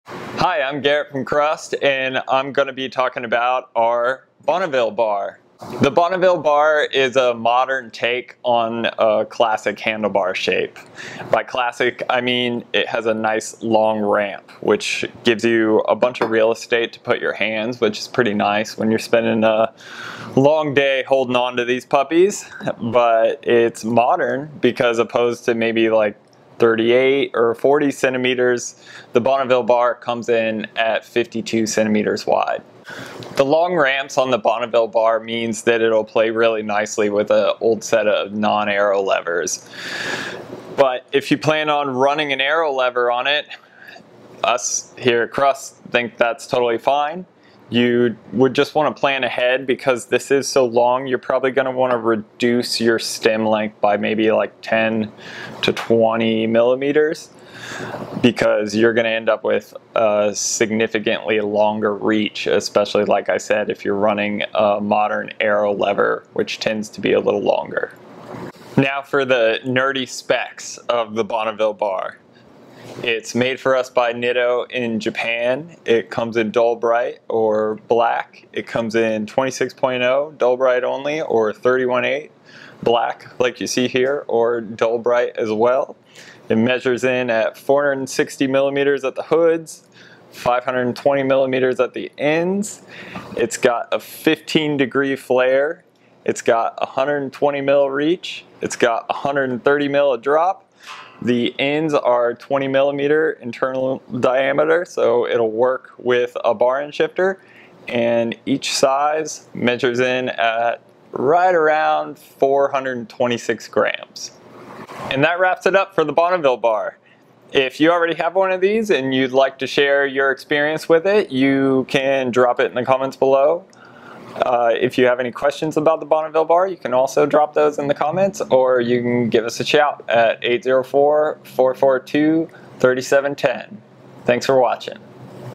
Bonneville Bar Nerd Talk
A nerdy chat with Garrett about our new long ramp'd rando bar made for us by Nitto in Japan.

If you want to check out the Bonneville bar in a bit more detail here's a link to that: